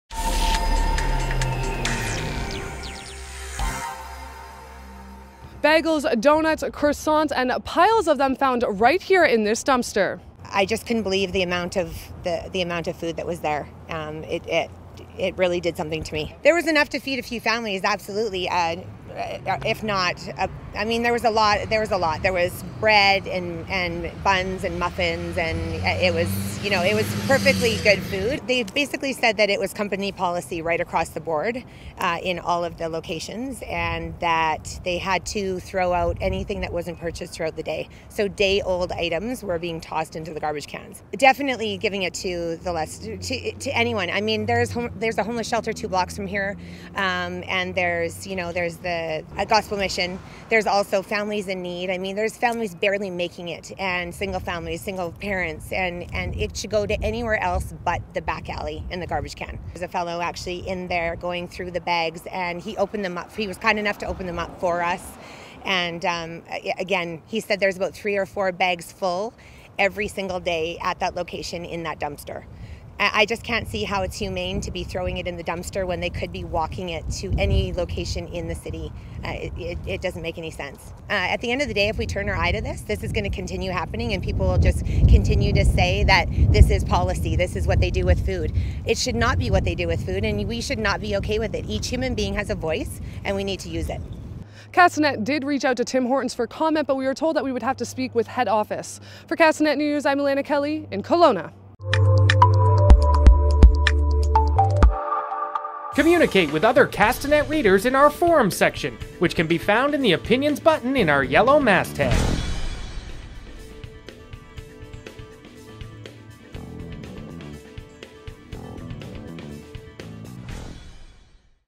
Bags full of food trashed
Tim Horton's was caught throwing bags full of food in the dumpster. (Video: Alanna Kelly)